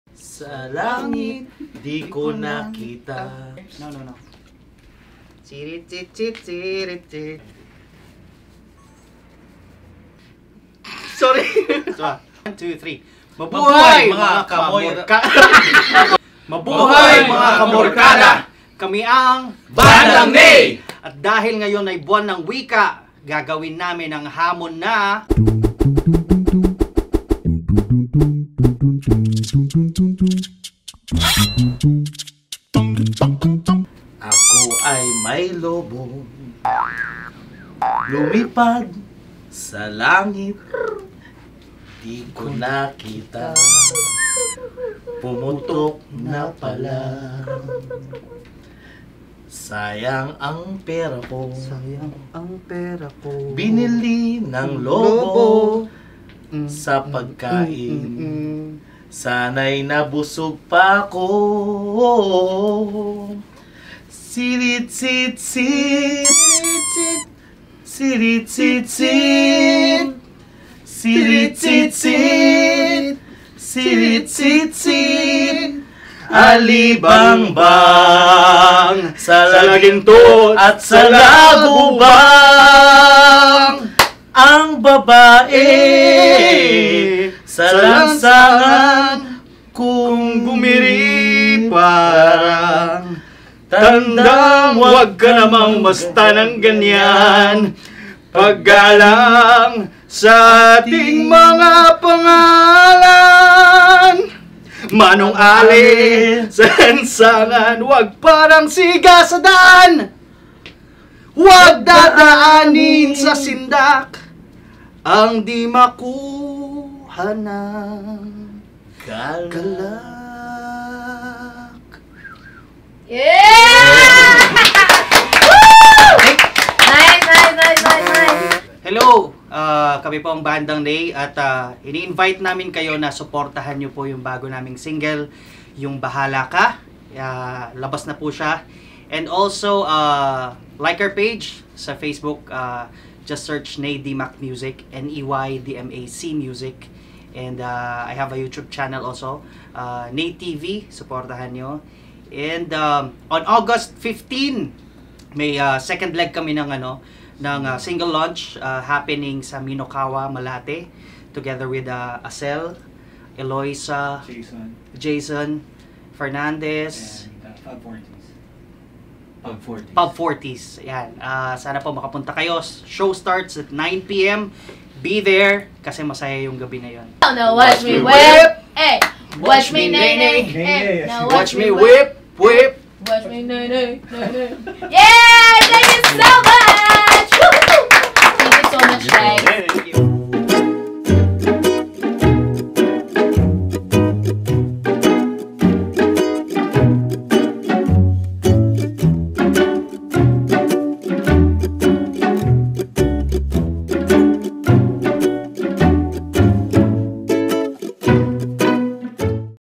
KANTA MO, ISTILO KO feat. BANDANG NEY (Buwan Ng Wika Challenge)
Nasaan na kaya yung lobo na dapat ibigay ni Ney sa kanyang pamangkin? Lumipad na ba?

Mga kaMORkada! Panuorin natin ang ating mga paboritong banda at artist na kumanta ng kanilang sariling bersyon ng mga folk songs. Ang kukulit!